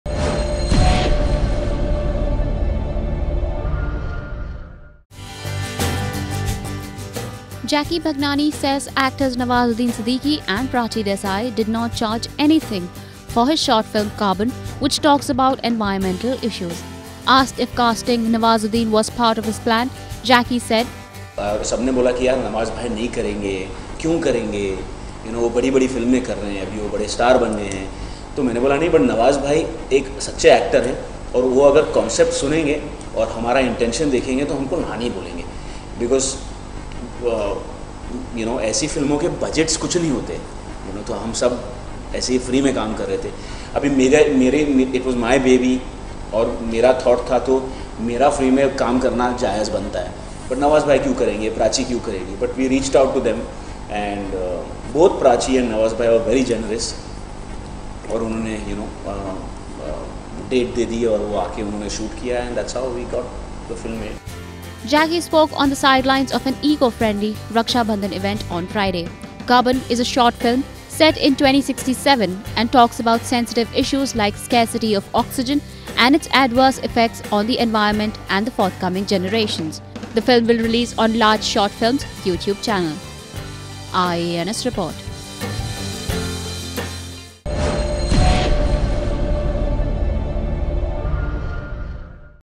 Nawazuddin-Prachi didn't charge for 'Carbon': Jackky
Jackky Bhagnani says actors Nawazuddin Siddiqui and Prachi Desai did not charge anything for his short film "Carbon", which talks about environmental issues. Asked if casting Nawazuddin was part of his plan, Jackky said here: "No, it was never a part of my plan.